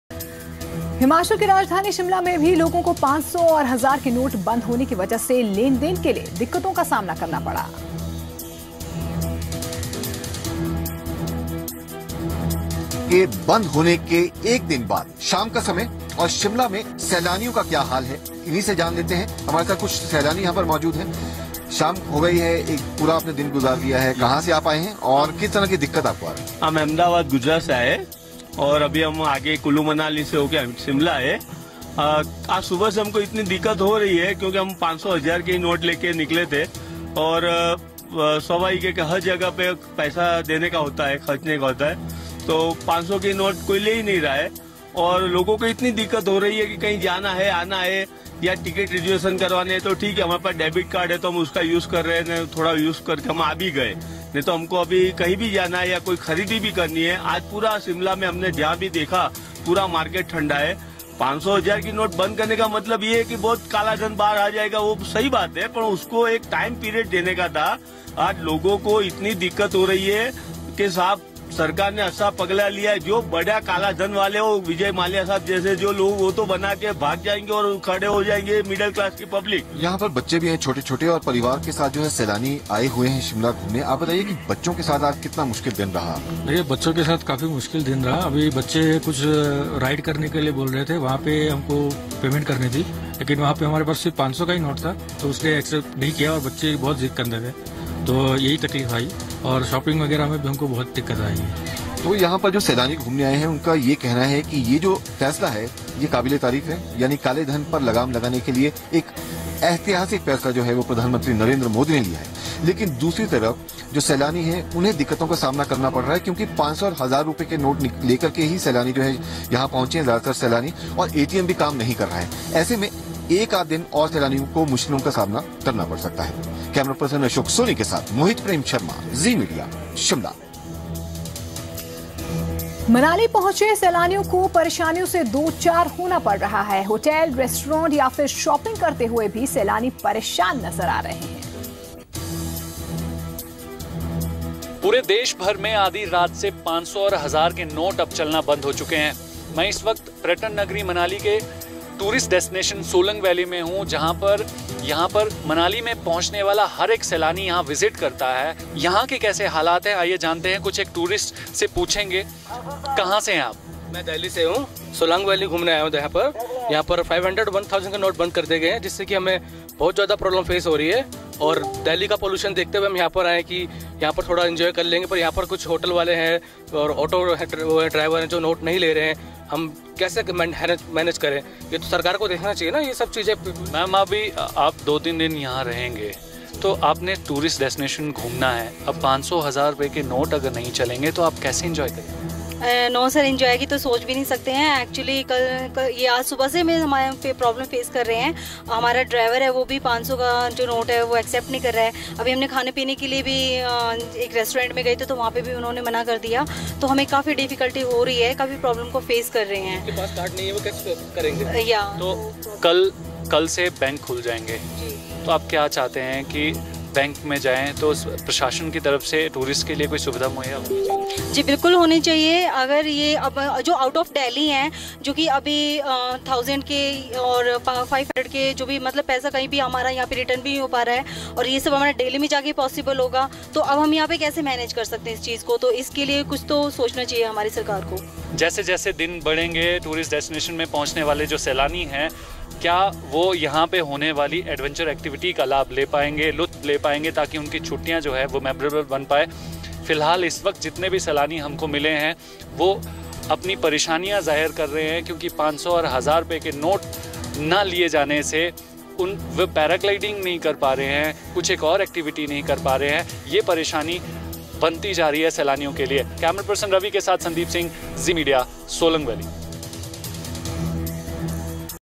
People in Shimla faced problems after discontinuing of 1000 and 500 notes
People in Shimla faced problems after discontinuing of 1000 and 500 notes. Watch complete news story for getting detailed updates!